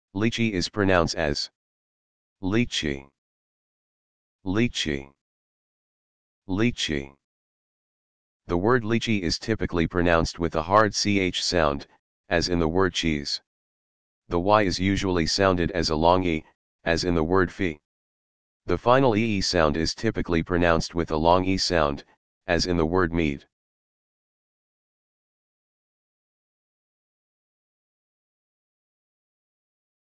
How To Pronounce Lychee
The word "lychee" is typically pronounced with a hard "ch" sound, as in the word "cheese." The "y" is usually sounded as a long "e," as in the word "fee." The final "ee" sound is typically pronounced with a long "e" sound, as in the word "meet."